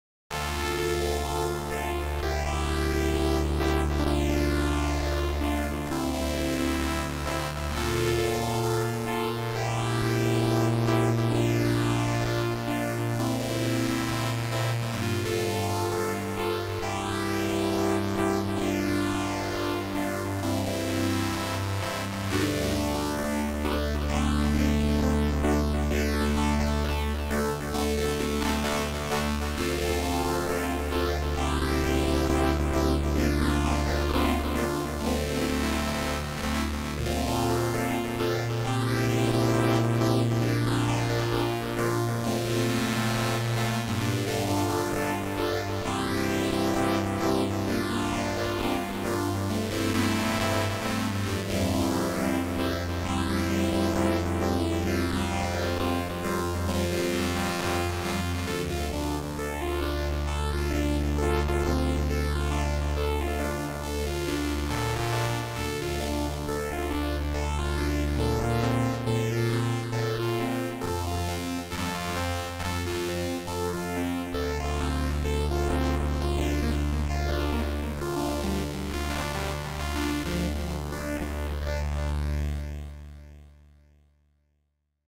DiY analog synth test
DIY analog poly synth. 36-voice polyphony.